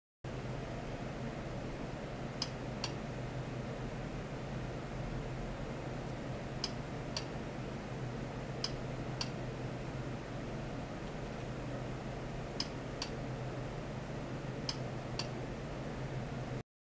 非線形現象の音圧測定解析に基づいた、超音波伝搬制御実験（表面弾性波の特性テスト）
１メガヘルツの超音波発振で、
　１００メガヘルツ以上の周波数変化を含めた

参考